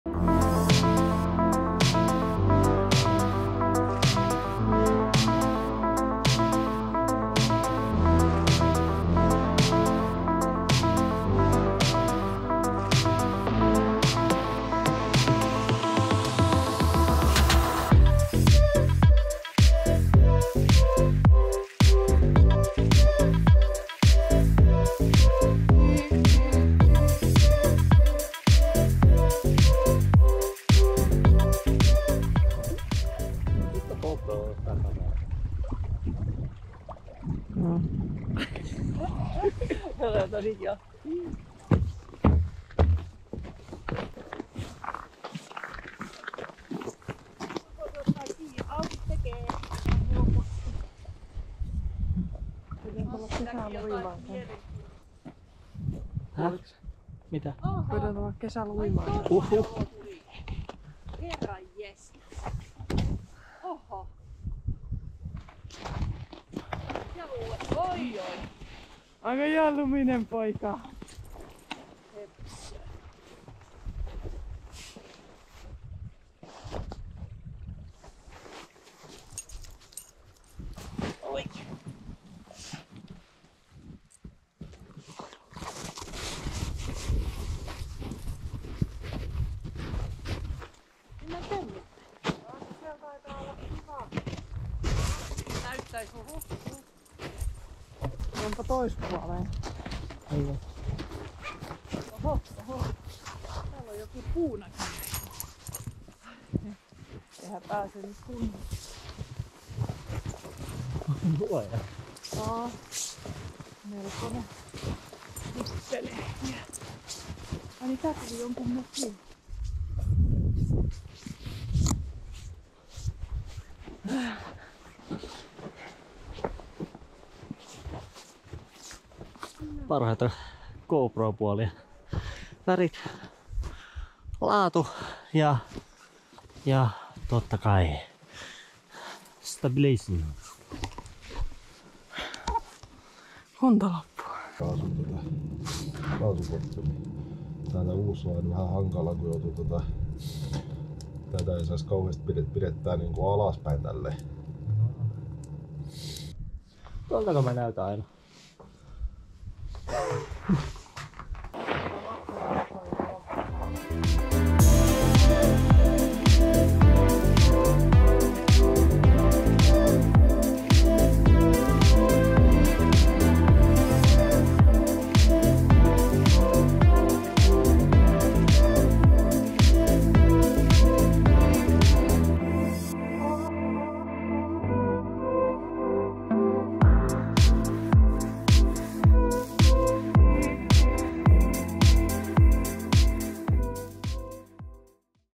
Makkaraa ja Anssin leikit | Varvojärvi
Lähdettiin viikonloppuna ulkoilemaan ja tein tälläsen pienen vlogin

TOIVOTTAVASTI TYKKÄÄT VIDEOSTA JA LAITAPPA KANAVA TILAUKSEEN JOS ET SITÄ VIELÄ OLE TEHNYT.